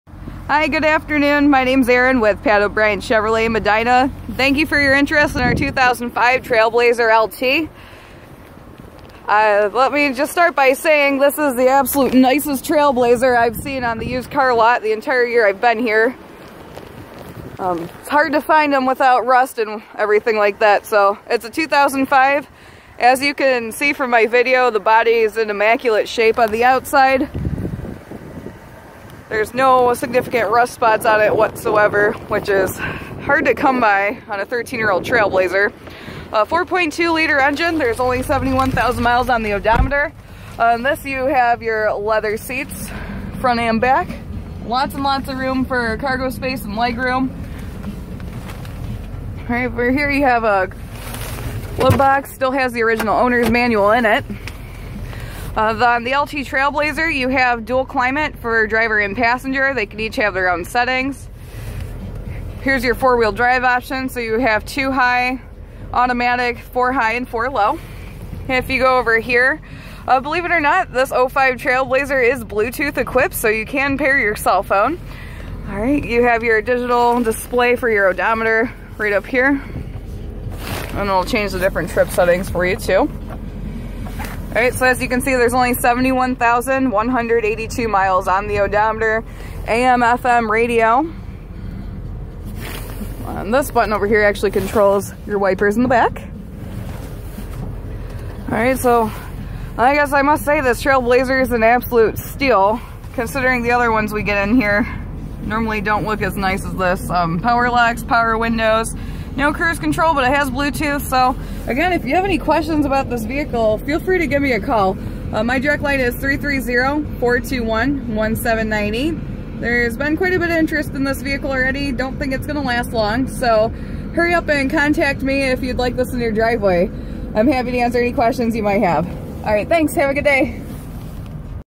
2005 Chevrolet Trailblazer LT
13 yr old trailblazer that's NOT rusted out. Holy cow check it out. Only 71k miles too.